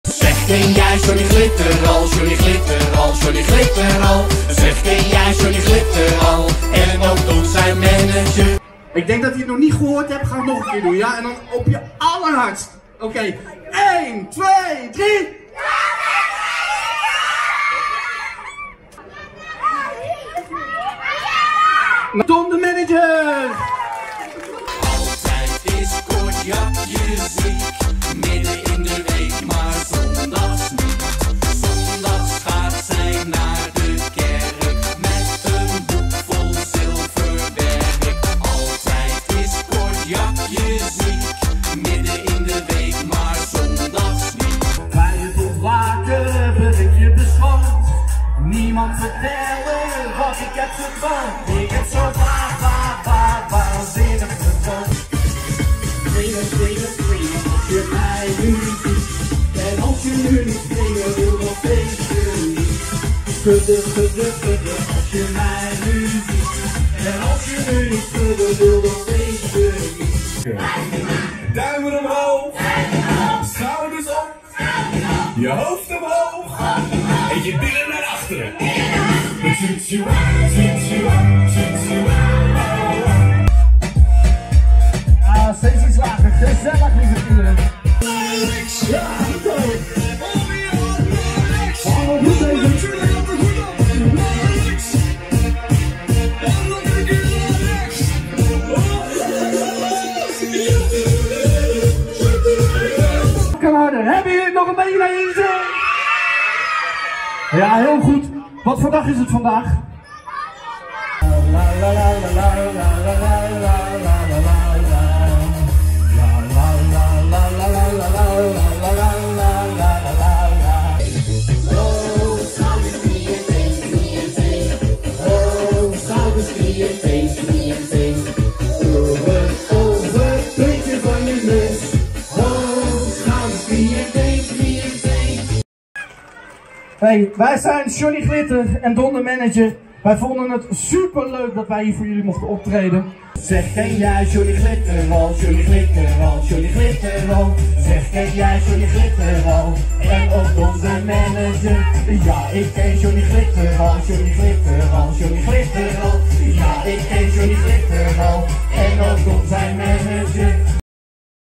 Johnny Glitter - Kinderdisco Promofilm
Kinderdisco of verjaardagsfeestje  gezocht of boeken met:
-zang
-DJ
-spelletjes
-dansjes
-Sneeuw / bellenblaasmachine
-weggevertjes

Geschikt voor bij een: Verjaardagsfeestje, speeltuin, evenement schoolfeest, braderie, kermis etc.